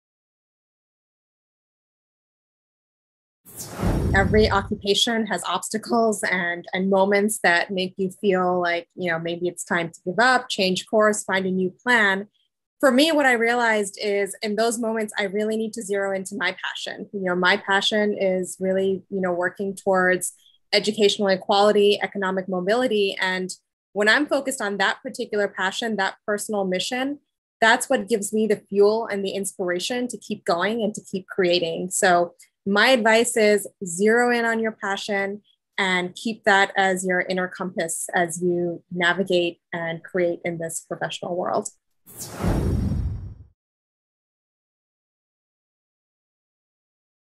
Advice to NextGen Women — Monoswita Saha, CSR Manager, Kyndryl
Girls With Impact called on America’s business leaders to share their advice with the next generation of women leaders. Monoswita Saha, CSR Manager at Kyndryl says to focus on zero-ing in on your passion to get through tough work.

Join us in training the next generation of women leaders.